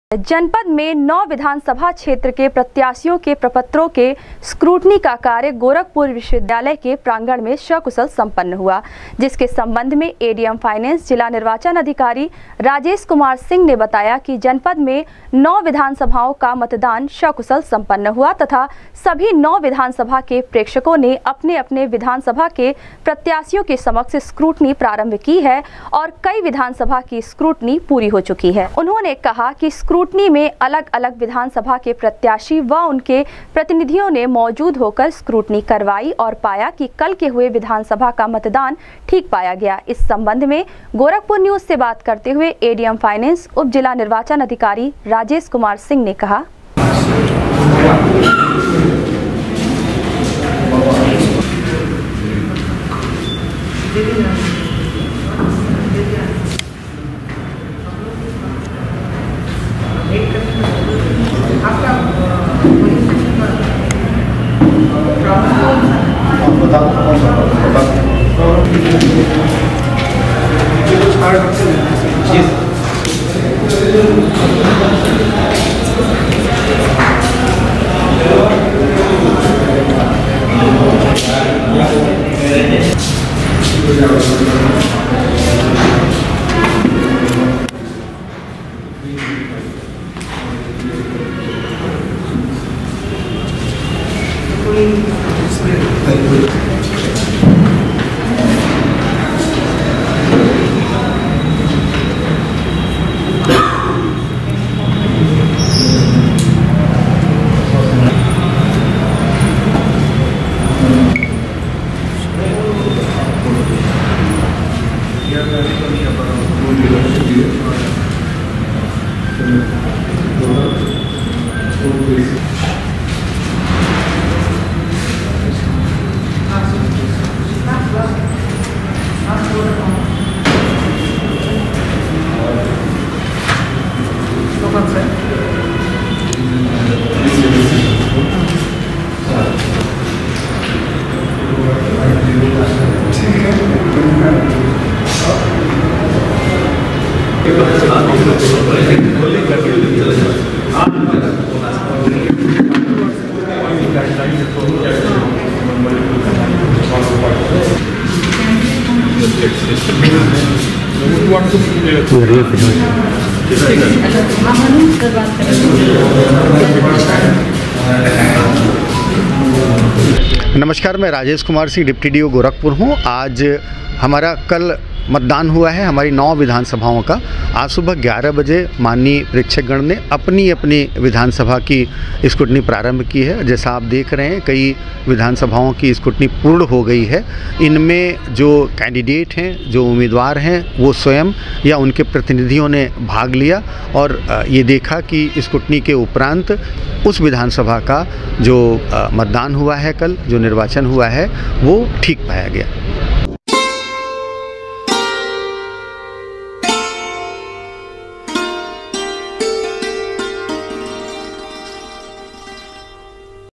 जनपद में 9 विधानसभा के प्रत्याशियों के प्रपत्रों के स्क्रूटनी का कार्य गोविवि के प्रांगण में सकुशल
जनपद में 9 विधानसभा के प्रत्याशियों के प्रपत्रों के स्क्रूटनी का कार्य गोविवि के प्रांगण में सकुशल हुआ संपन्न,एडीएम फाइनेंस/उपजिलानिर्वाचन अधिकारी ने कहा, प्रत्याशियों व उनके प्रतिनिधियों के निगरानी में की गयी प्रेक्षकों के समक्ष स्क्रूटनी ।।